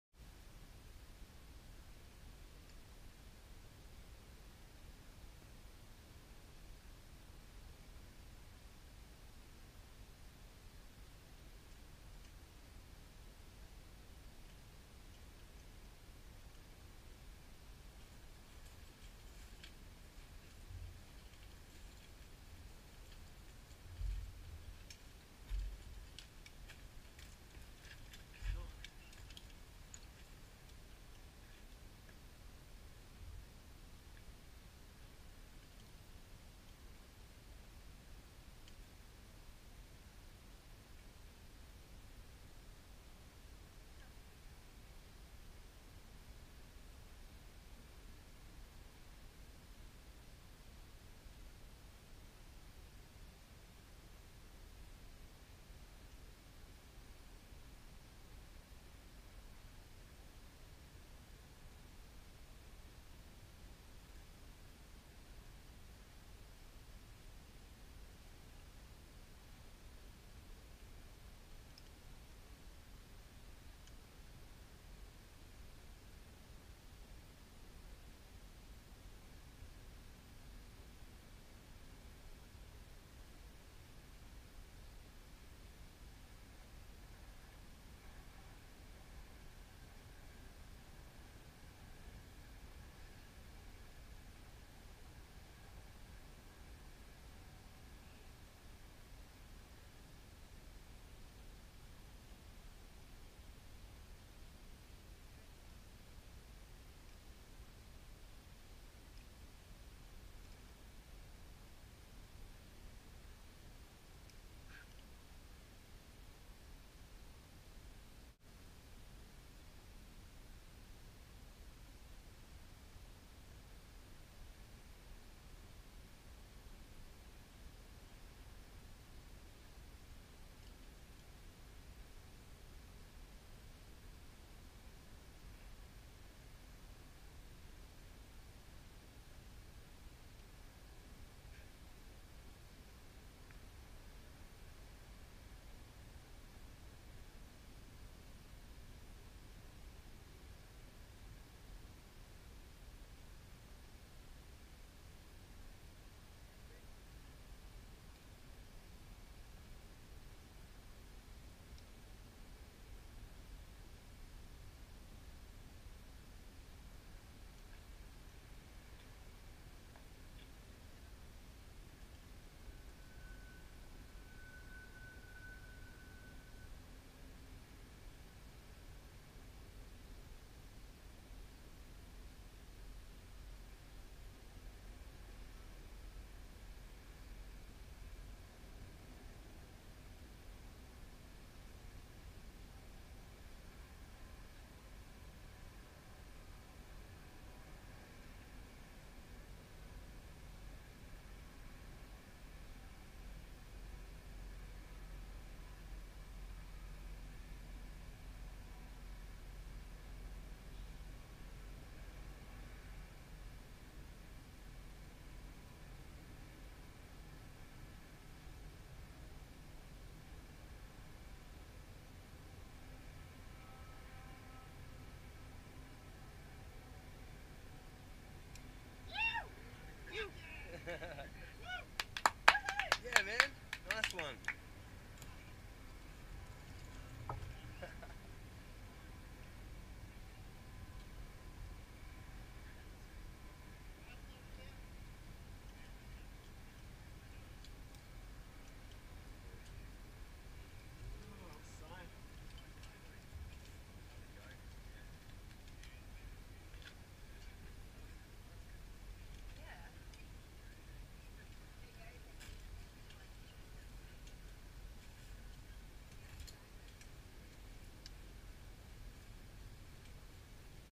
Send for Ken (grade 20) - Full GoPro footage.
First free ascent of 'Send for Ken'.  A climb on The Escarpment in the Illawarra.